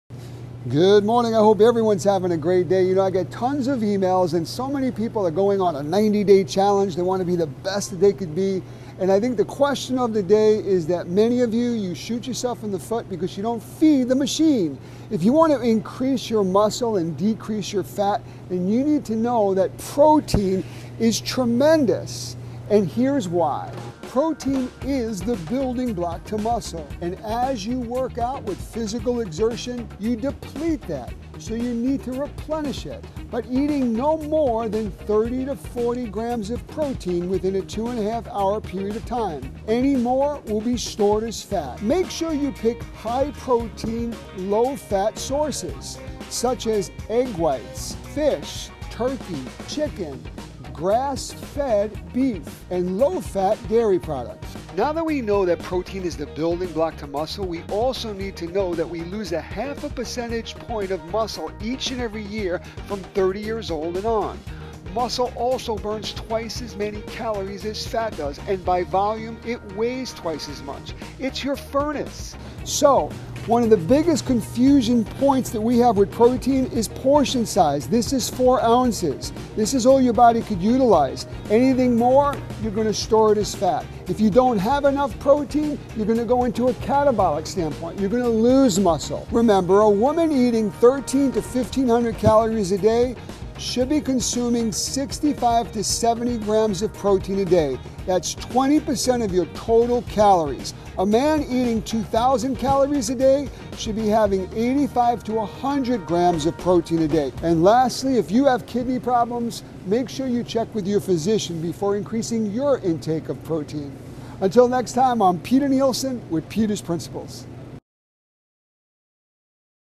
The importance of protein to lose fat or gain muscle
Health expert Peter Nielsen explains why protein in your eating plan is so important for you